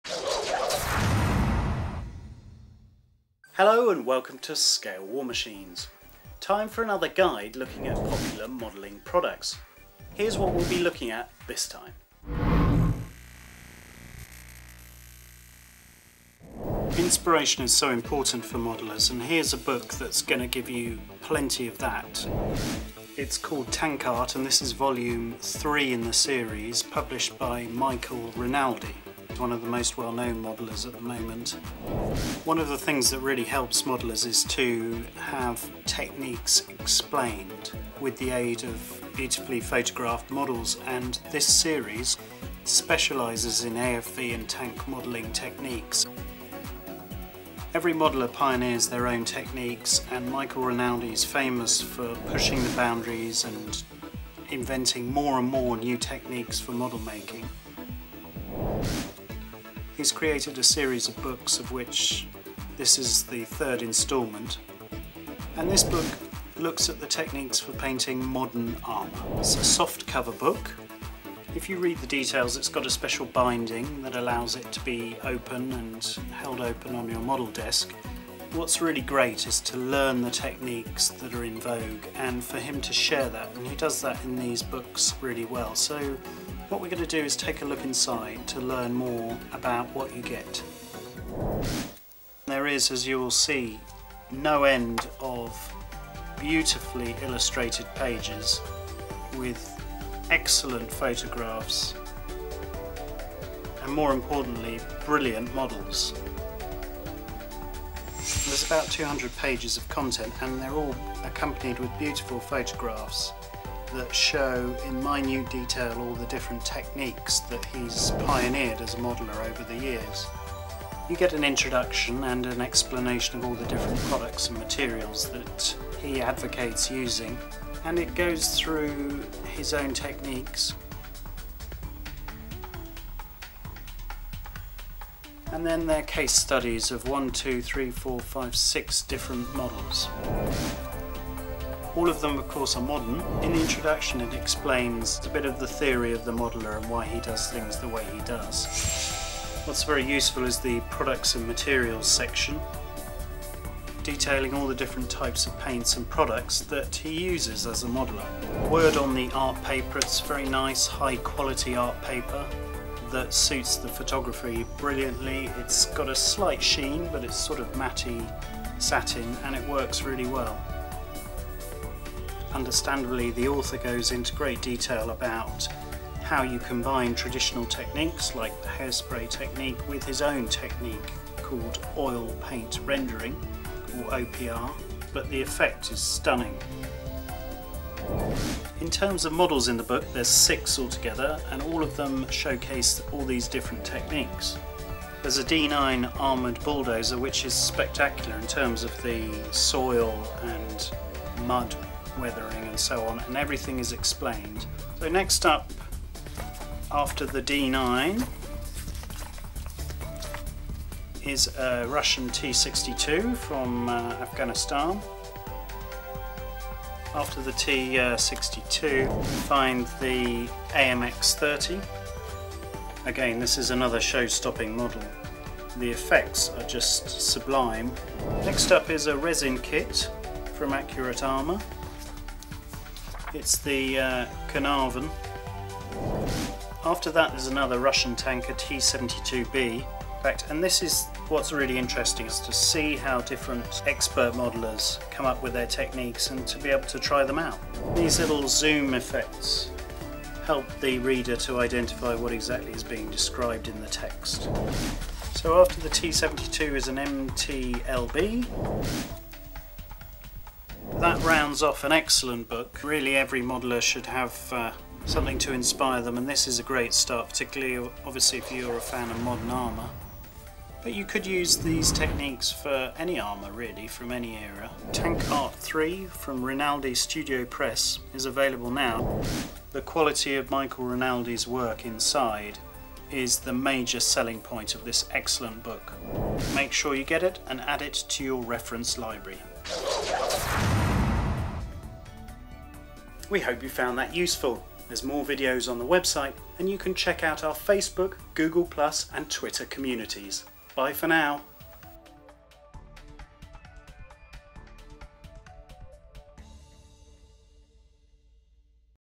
TANKART VOLUME 3 BY MICHAEL RINALDI
A review of Tankart Volume 3 - Modern Armour - the home of model making videos, articles & archive films.